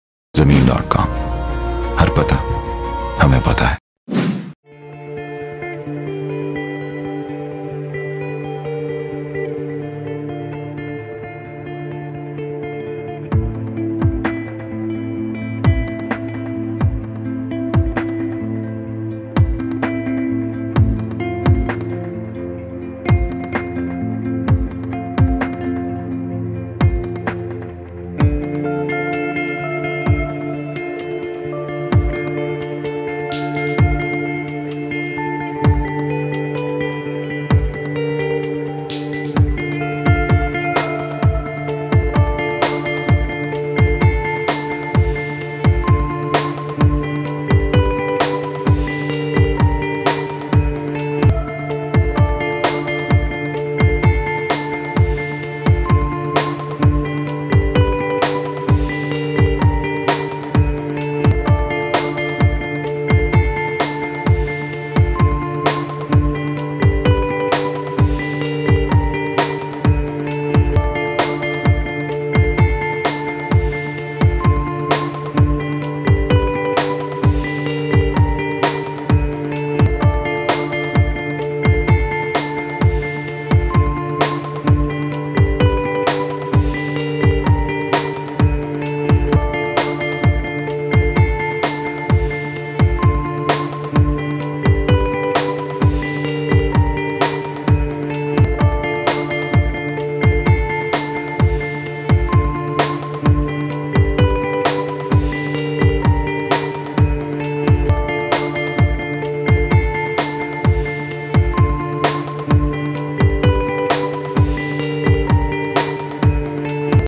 5 MARLA HOUSE FOR SALE AT OLD BARA ROAD PESHAWAR
New Fresh House For Sale
Hadi Line
Size: 5 Marla
Having:
8 Bed Attach Bath
3 Story Full Basement
2 Car Porch
Double Gate
Best Option For Living Purpose.
Good Location.
Reasonable Price.